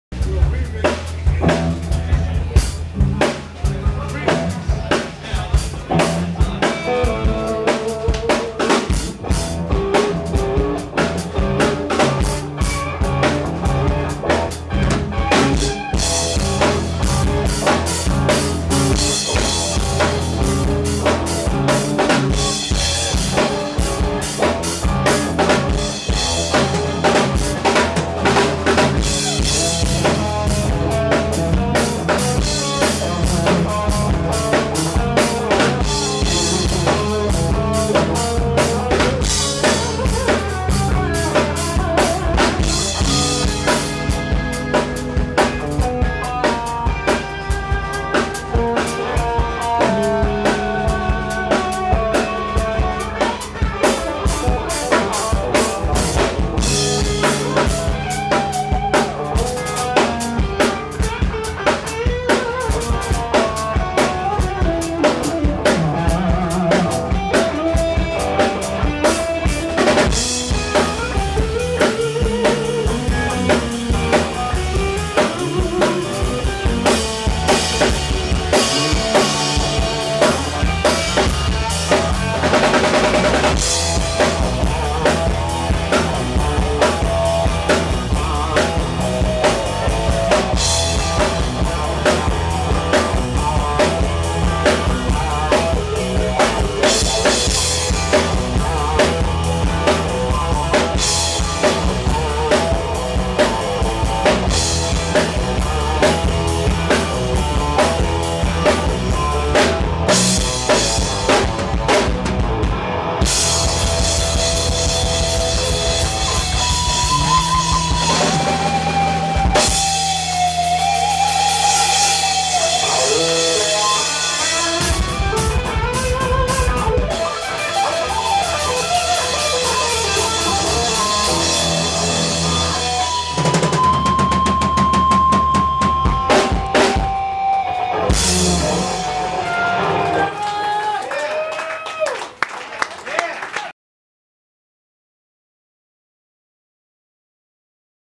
Larry Mitchell 10/12/2012
The incomparable Larry Mitchell *HD* (click the settings wheel to 1080) at the Zebra Club in Copiague, NY on 10/12/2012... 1 of 5